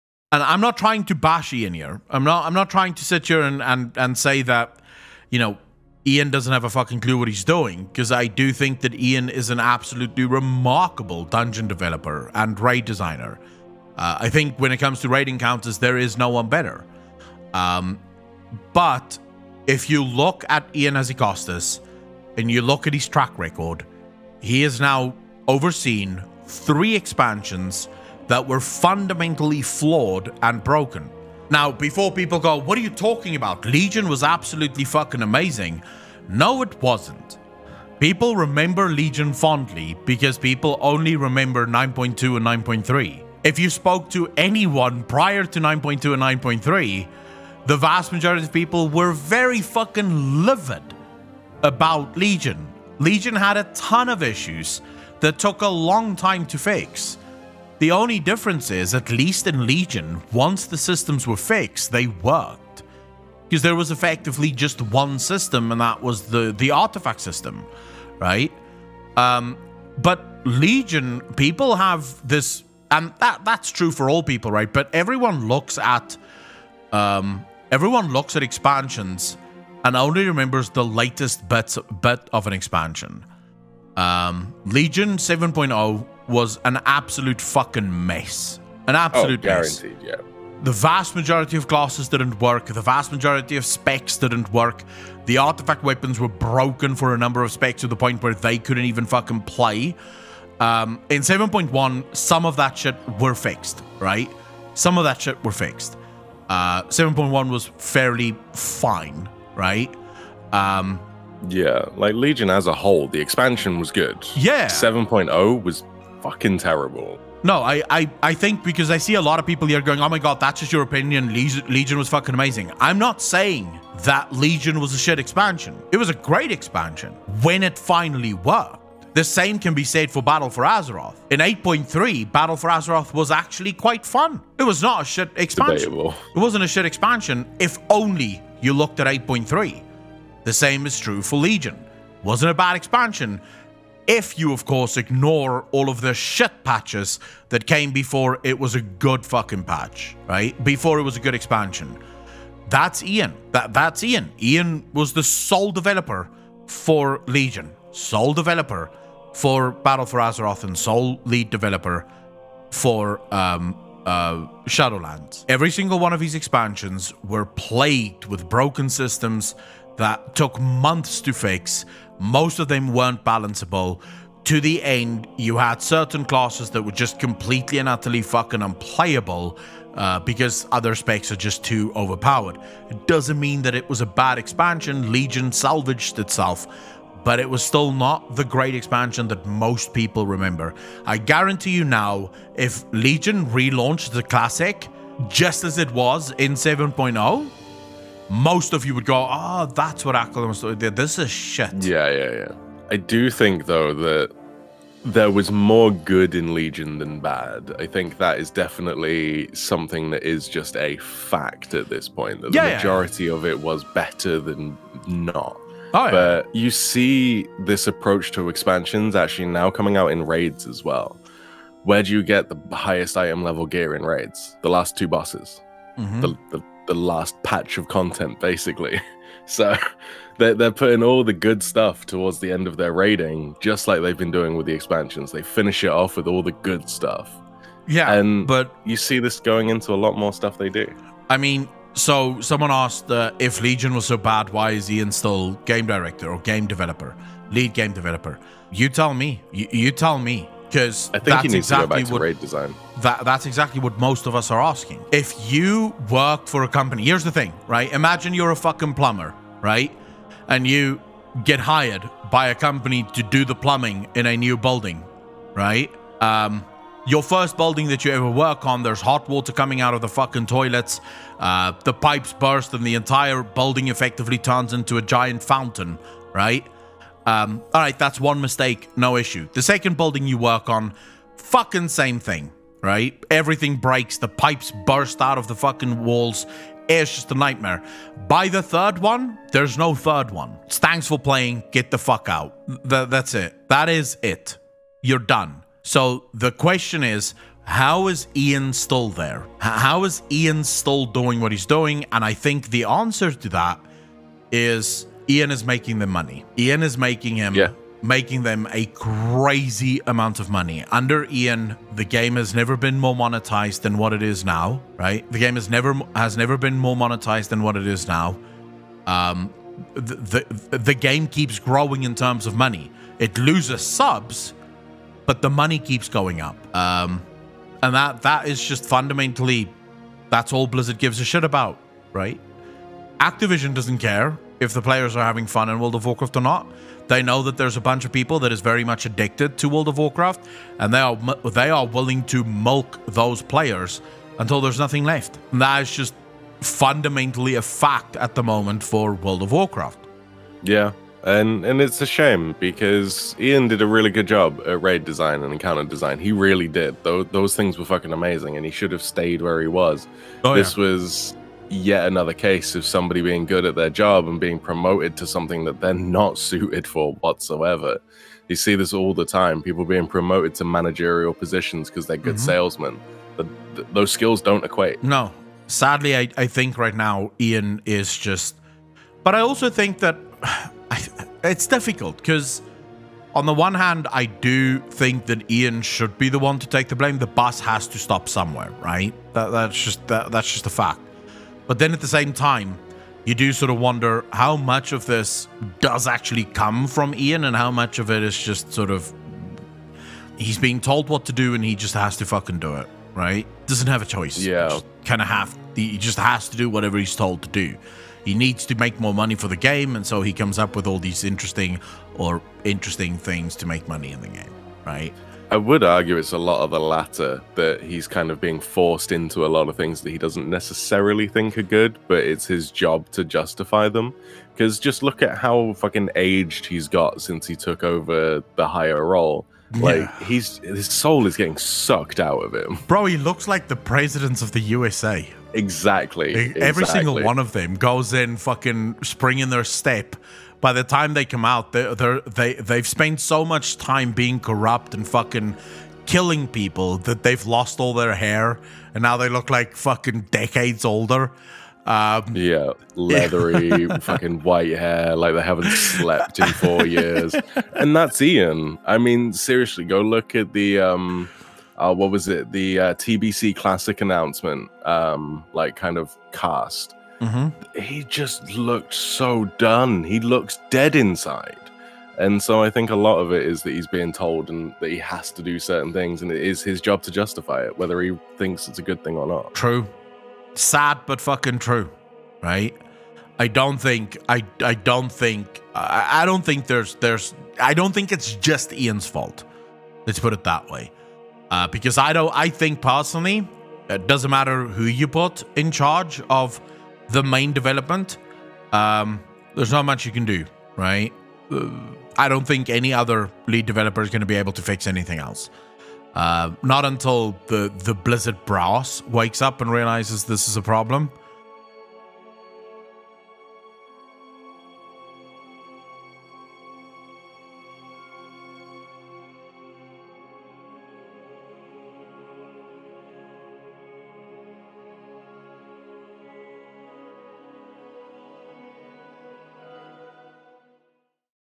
Ion Has a Lot to PROVE With Shadowlands 9.1.5!
Ion and the Blizzard Developers have a lot to prove with World of Warcraft Shadowlands Patch 9.1.5. Are they even up to it?